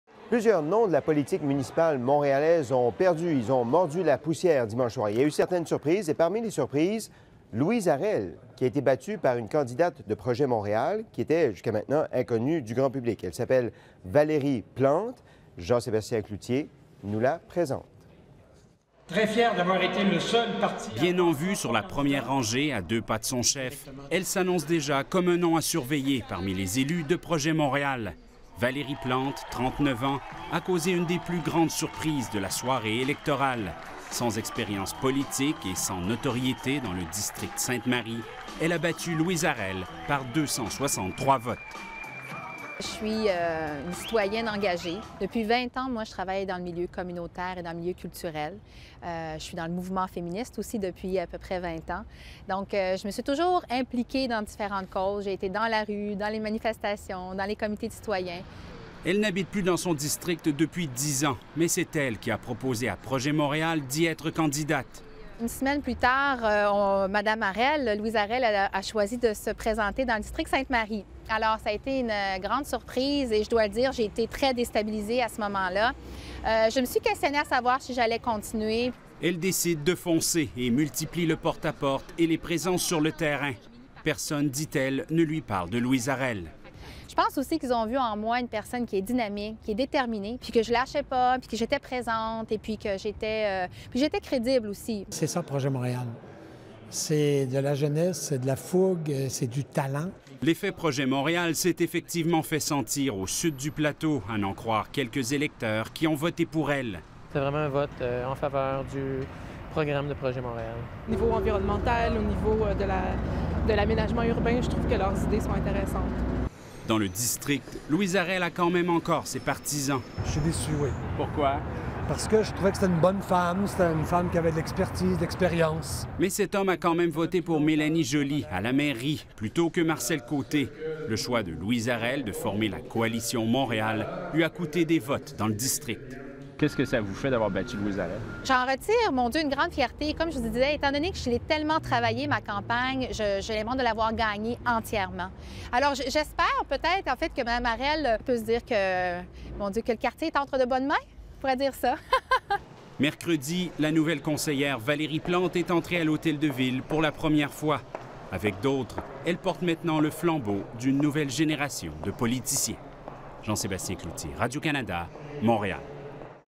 La nouvelle génération
Plusieurs noms de la politique municipale montréalaise ont mordu la poussière dimanche soir. Parmi eux, Louise Harel, qui a été battue par une candidate de Projet Montréal, inconnue du grand public.  Elle s'appelle Valérie Plante et Jean Sébastien Cloutier nous la présente.